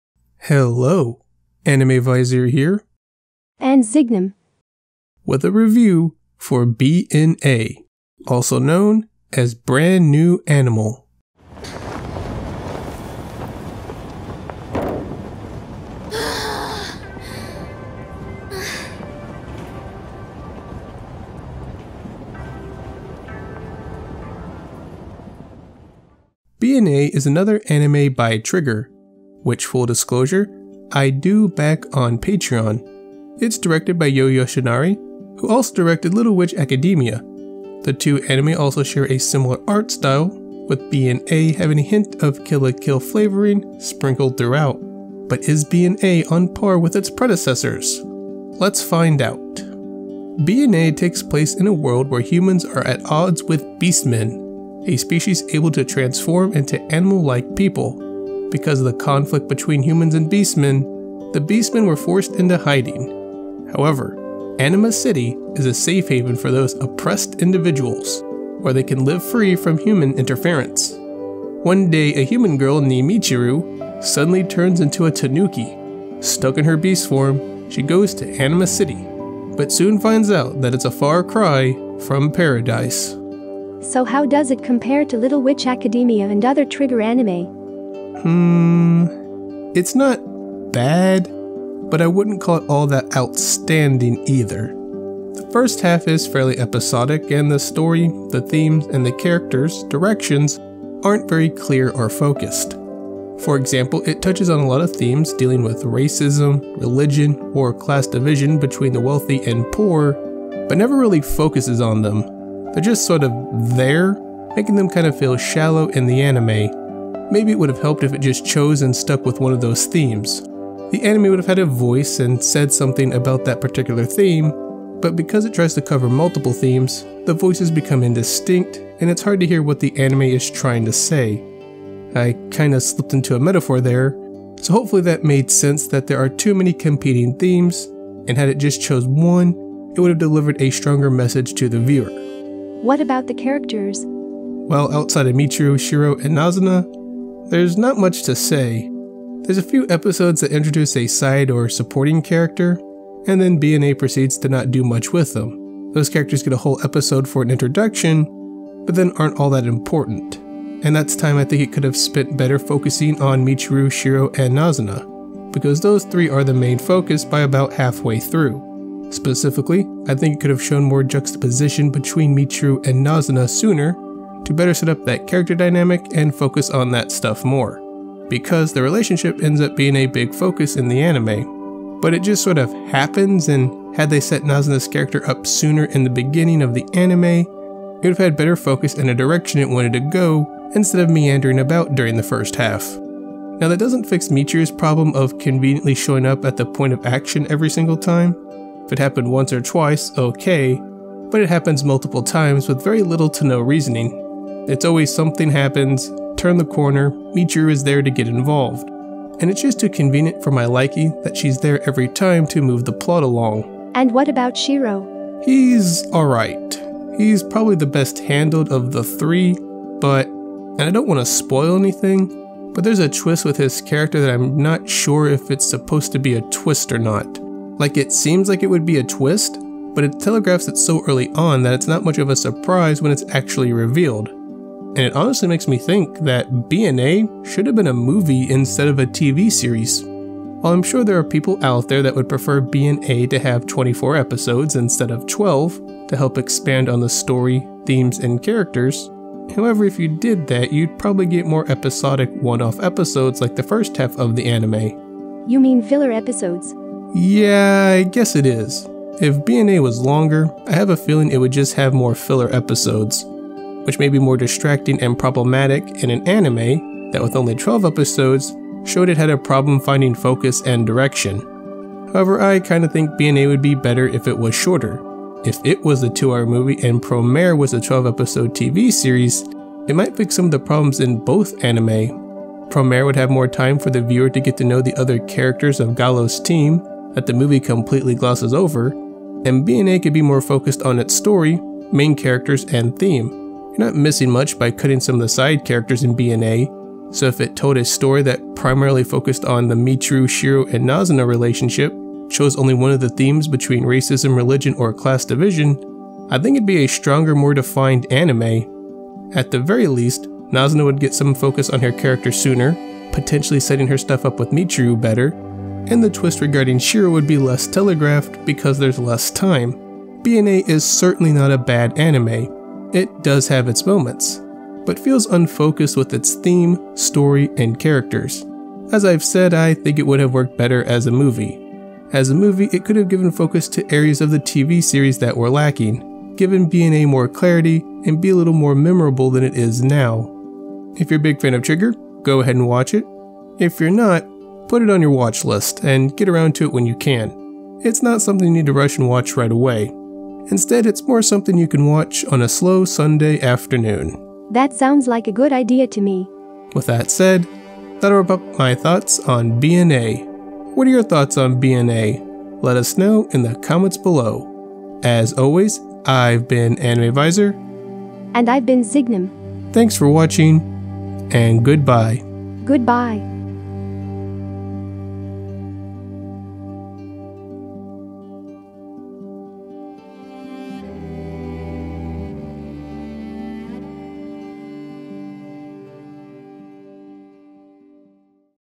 BNA: Brand New Animal Review | Not Bad, But Unfocused
Trigger's newest anime. Join SiGNUM and myself to see how it stacks up against it's predecessors in this review.